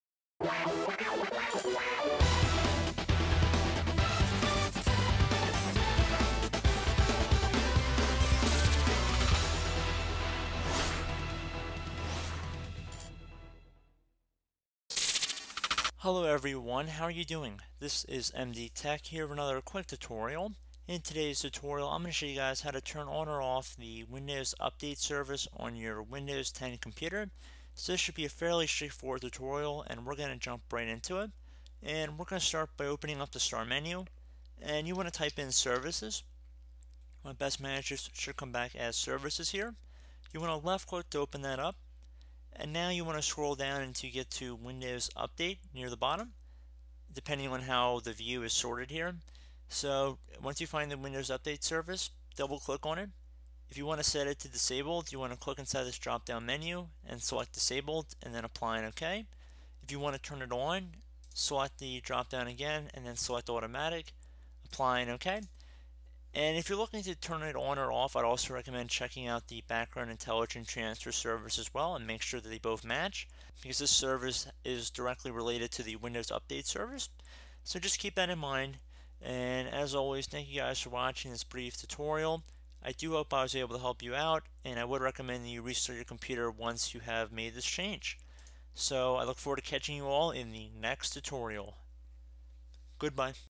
Fix Windows Update Service Couldn't Stopped (Solve All the Problem in Windows 10)
Microsoft Says Windows 10 will be always up to date. There is no way to turn windows 10 automatic update. However we have another simple option to disable windows 10 automatic update.